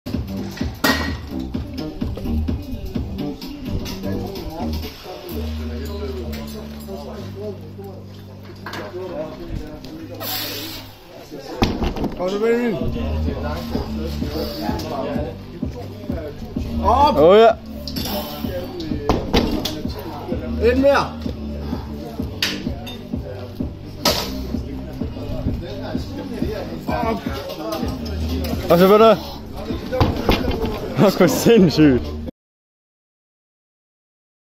220KG 485 lbs Deadlift 15 year old 3 REPS
maybe i can get 240 now, but i will wait a bit.